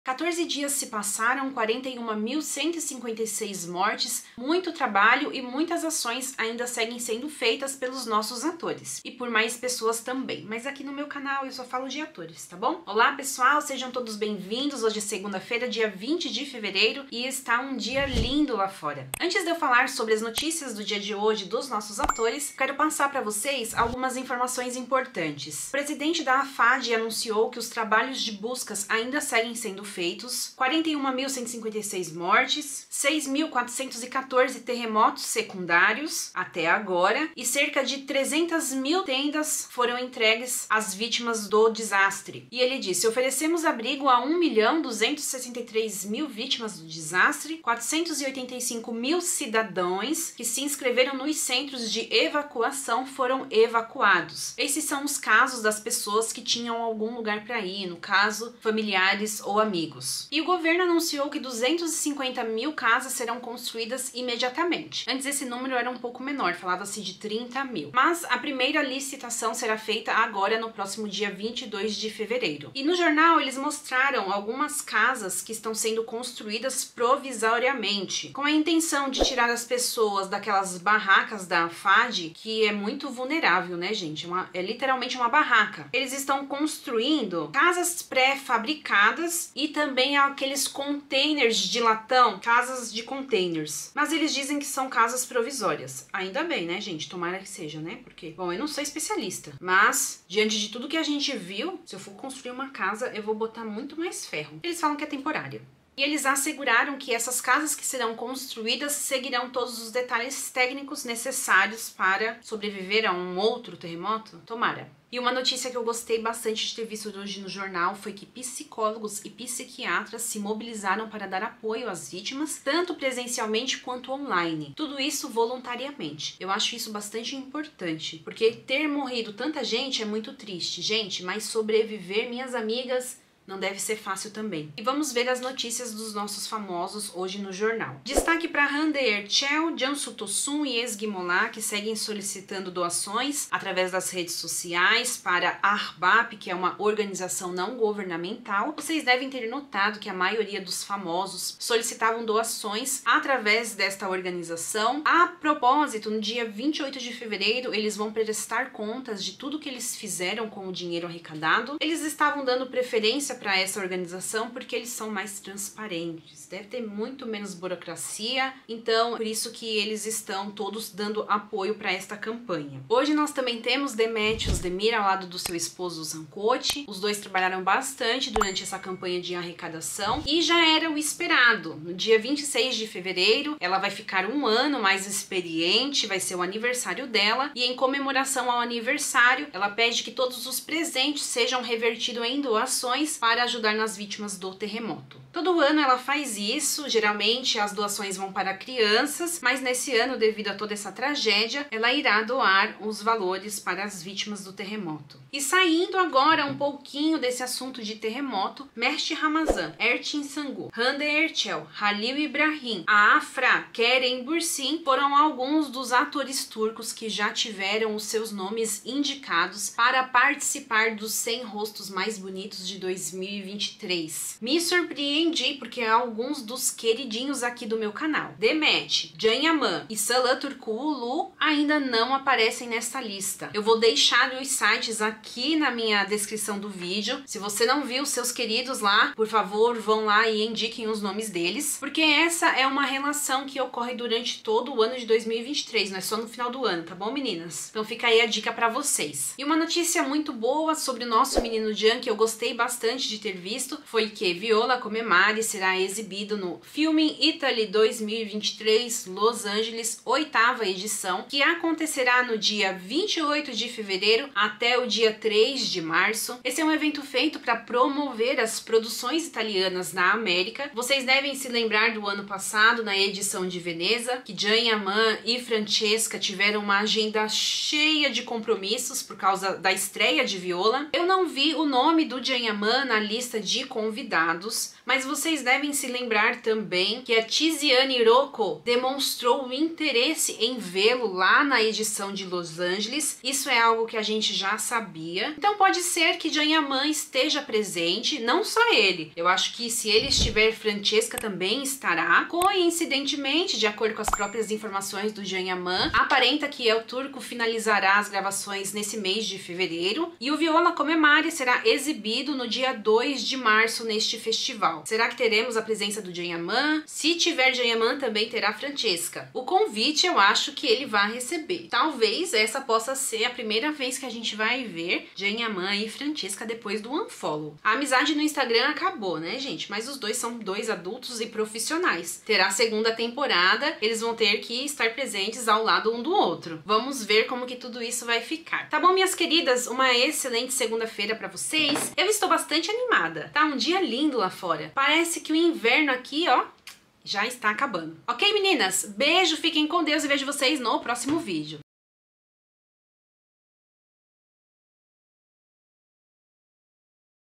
🔴 uma possibilidade para Can Yaman, noticias Demet Ozdemir, Hande e atualização terremoto na Turquia
Um ato entre todos. Juntos pela Turquia.

🌺Olá, eu sou a Denise e o meu Instagram é o @vivendonaturquia.